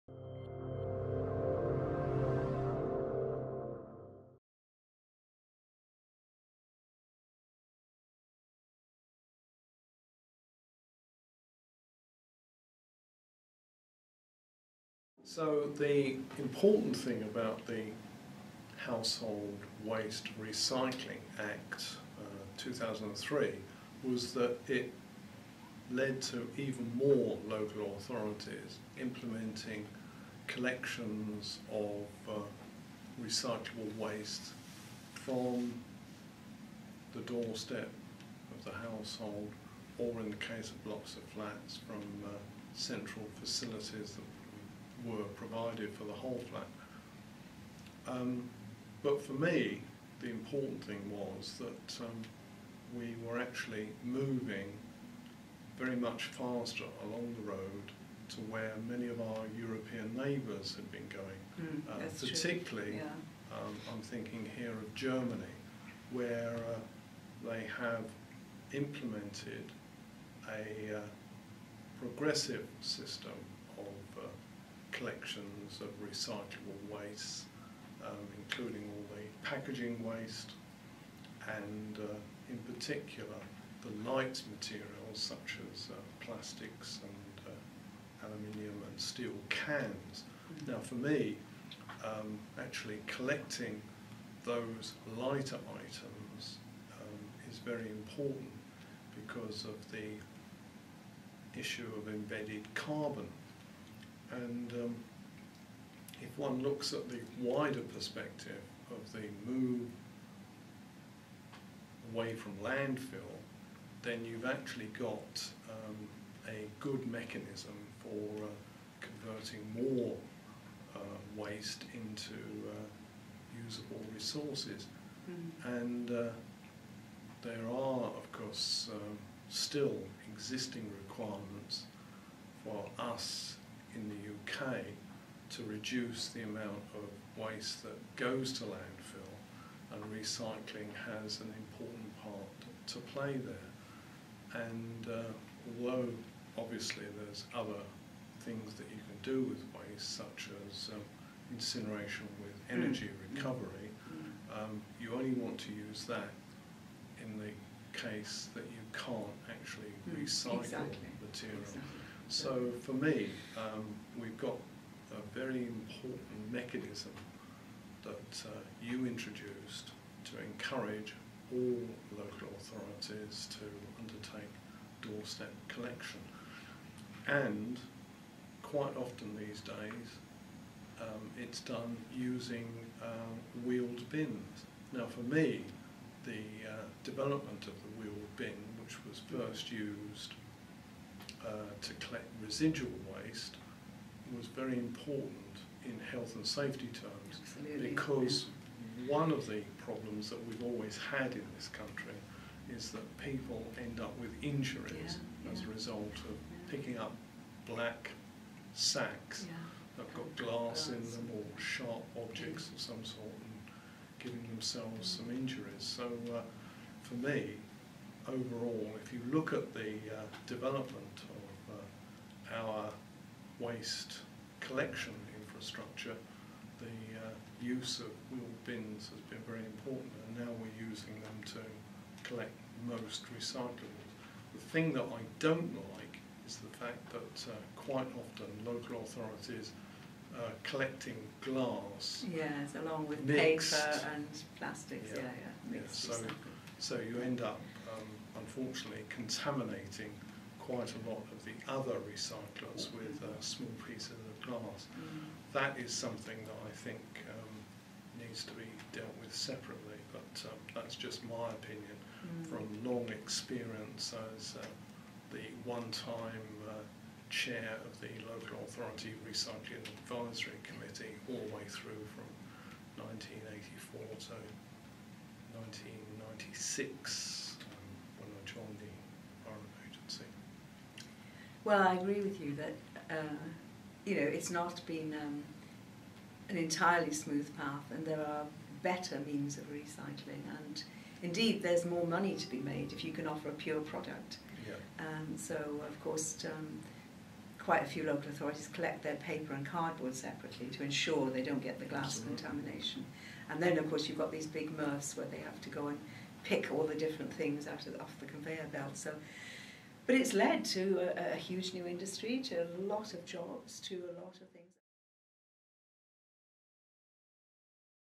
Ruddock, Joan and Cooper, Jeff 22 The Household Waste Recycling Act 2003; health and safety impact
Interview conducted by Ms Lynda Finn, with additional questions by Mr Alan Yabsley, for the History of Modern Biomedicine Research Group, 07 June 2016, in the School of History, Queen Mary University of London. Transcribed by Mrs Debra Gee, and edited by Mr Alan Yabsley and Professor Tilli Tansey.

Dame Joan Ruddock DBE BSc ARCS (b. 1943) was a Member of Parliament (MP) for Lewisham Deptford from 1987 to 2015. In 1989 she brought forward a Private Members’ Bill on fly-tipping that became the Control of Pollution Act 1989 and, in 2003, a Private Members’ Bill that became the Household Waste Recycling Act 2003. In 2007 she became Parliamentary Under Secretary in the Department for Environment, Food and Rural Affairs and requested the waste brief, serving as Minister for Biodiversity, Climate Change and Waste until she joined the newly created Department for Energy and Climate Change in October 2008. Dame Joan gave her final speech to the House of Commons on 26 March 2015.

Mr Jeff Cooper MSc (b. 1949) became Waste Recycling Coordinator at the Greater London Council in 1982. He was subsequently appointed Waste Planner for the London Waste Regulation Authority (LWRA) where he also represented the International Solid Waste Association’s recycling working group as its Vice-Chair. He joined the newly-formed Environment Agency in 1996 on a project for the development of regulations for packaging waste. He was elected Junior Vice President of the Chartered Institution of Wastes Management in 2004, and served as President from 2007 to 2008. Since 2009 he has worked as an independent consultant and journalist.

Citation of the current clip: Finn L, Yabsley A (intvrs); Yabsley A (prod) (2016) Ruddock, Joan & Cooper, Jeff: 22 - The Household Waste Recycling Act 2003; Health and Safety impacts (07-Jun-2016). History of Modern Biomedicine Interviews (Digital Collection), item e2016159. London: Queen Mary University of London.